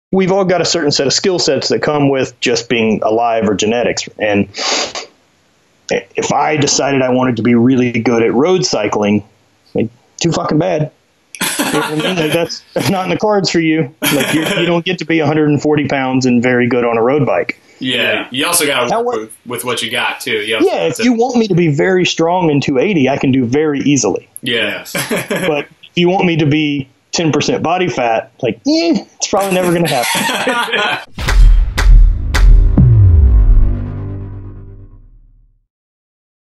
Respect your genetics! (Matt Vincent - Ep. #37)
Highlight from episode #37 featuring Matt Vincent!

MATT’S INSTAGRAM: @ihviiimattvincent
HVII BRAND INSTAGRAM: @hviiibrandgoods

Love the show? Make sure you leave us a five star review!